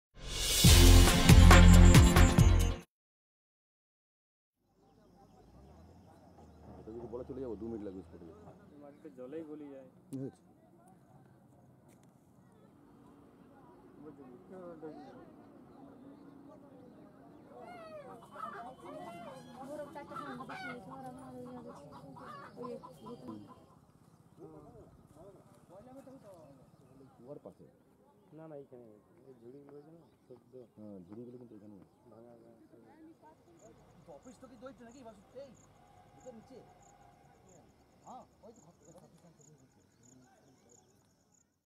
3 died in a landslide at coal a mine in Asansol
3 died in a landslide at coal a mine in Asansol